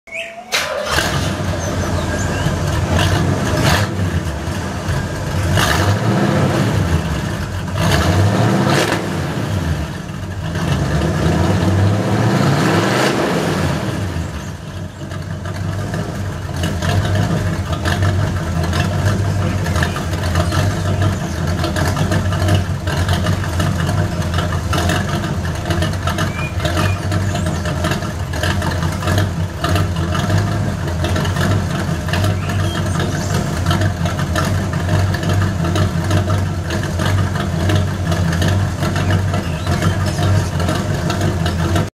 Buick idle and rev
Headers and straight pipes, no mufflers.
Stupid bird